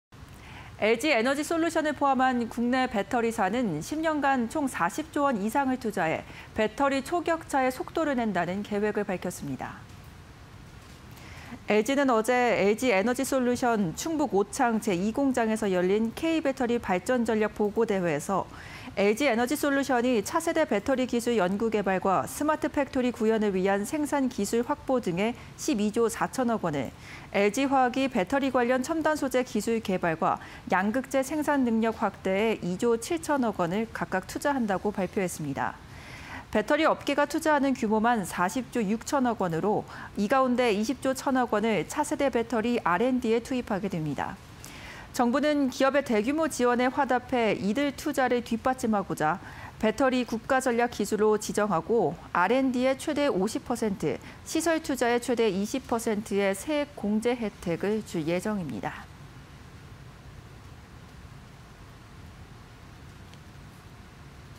[이데일리N] 정부, K배터리 ‘초격차’ 지원… LG 등 배터리사 40조 투자 (20210709)
LG에너지솔루션을 포함해 삼성SDI와 SK이노베이션 등 배터리사는 10년간 총 40조원 이상을 투자해 배터리 초격차에 속도를 낸다는 계획을 밝혔습니다
LG는 8일 LG에너지솔루션 충북 오창 제2공장에서 열린 ‘K-배터리 발전 전략 보고대회’에서 LG에너지솔루션이 차세대 배터리 기술 연구개발과 스마트 팩토리 구현을 위한 생산기술 확보·생산라인 증설 등에 12조4000억원을, LG화학이 배터리 관련 첨단소재 기술 개발과 양극재 생산능력 확대에 2조7000억원을 각각 투자한다고 발표했습니다.
배터리 업계가 투자하는 규모만 40조6000억원으로 이 가운데 20조1000억원을 차세대 배터리 R&D에 투입하게 됩니다.
정부는 기업의 대규모 지원에 화답해 이들 투자를 뒷받침하고자 배터리를 국가전략기술로 지정하고, R&D에 최대 50%, 시설투자에 최대 20%의 세액공제 혜택을 줄 예정입니다.